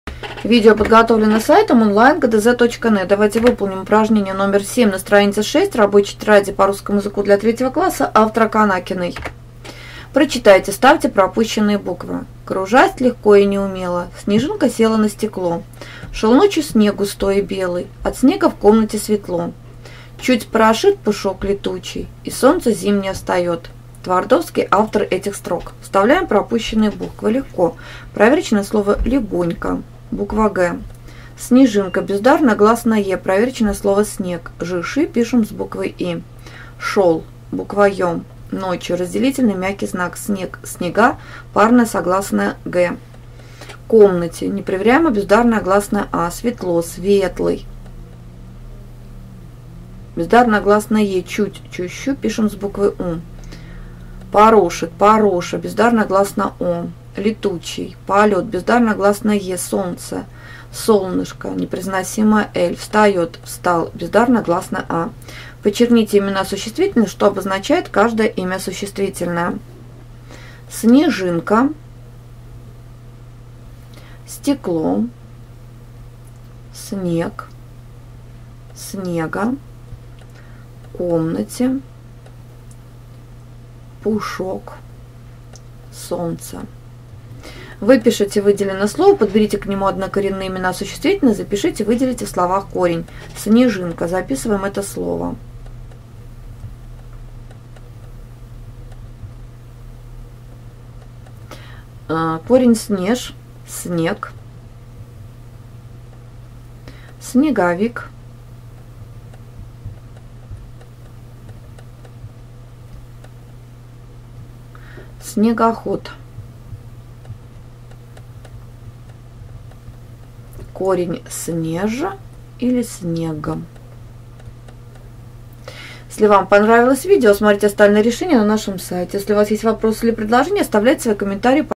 Упражнение 7 - ГДЗ по Русскому языку Рабочая тетрадь 3 класс (Канакина, Горецкий) Часть 2
Выполняя задание №7 на странице 6 вам нужно уметь вставлять в слова пропущенные буквы, подчеркивать имена существительные, к выделенному слову подбирать и записывать однокоренные имена существительные, а также выделять в них корень. Посмотрев наше видео, за 2 - 3 минуты можете выполнить данное задание.